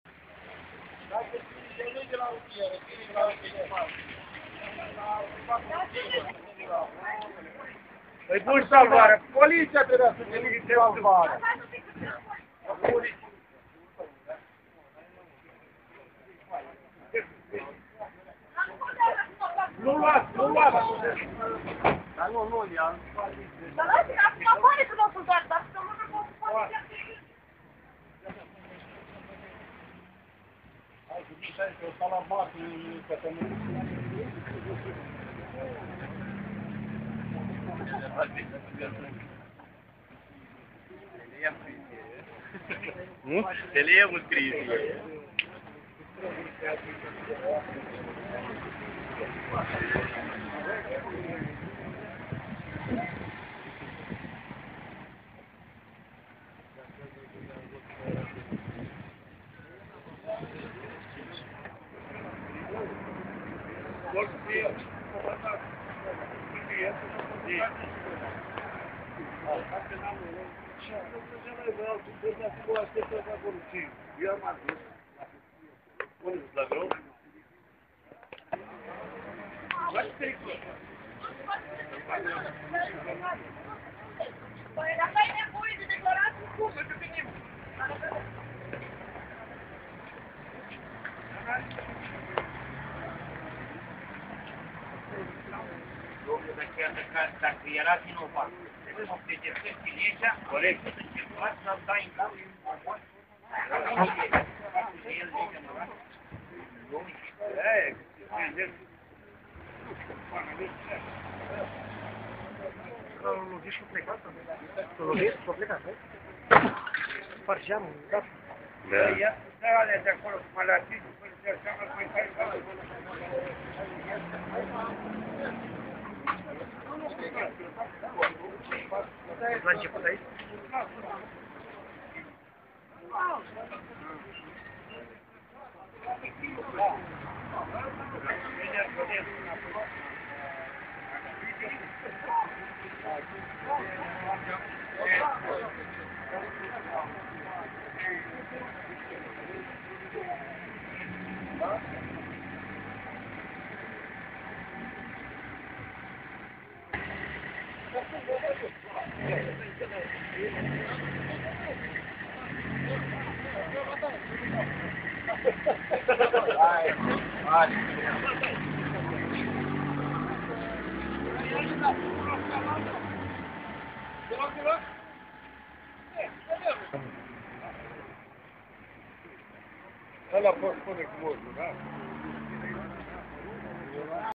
INCIDENT VIOLENT INTR-O INTERSECTIE DIN BOTOSANI-17.06.2010.vob
IN URMA UNOR NEINTELEGERI (ACORDARE DE PRIOR.)DIN TRAFIC UN SOFER SA DAT JOS DIN MASINA SI IA SPART GEAMUL ALTUI SOFER,RANINDUL PE FATA CU CIOBURILE REZULTATE DIN SPARGERE.FAPTASUL SA URCAT IMEDIAT LA VOLANUL MASINEI SALE SI A FUGIT DE LA LOCUL INCIDENTULUI.POLITIA A PLECAT IN URMARIREA AGRESORULUI.CULMEA E CA DINTRE TOTI TOTI MARTORI-ROMANI AFLATI ACOLO,A DEPUS MARTURIE UN...ITALIAN CEILALTI ROMANI NU AU AVUT CURAJ.17.06.2010